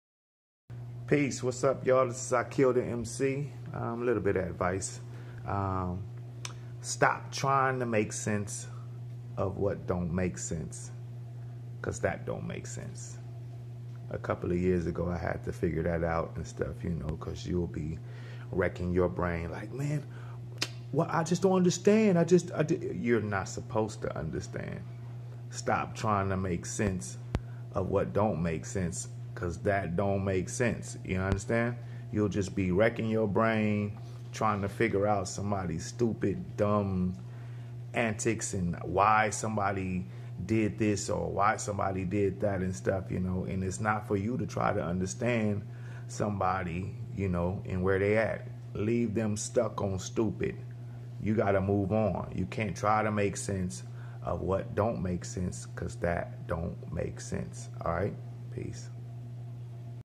Akil The Mc Advice: It Don't Make Sense or Cents
Akil Advice: Stop Trying to make sense of what Don't Make Sense Cause that Don't Make Sense or Cents.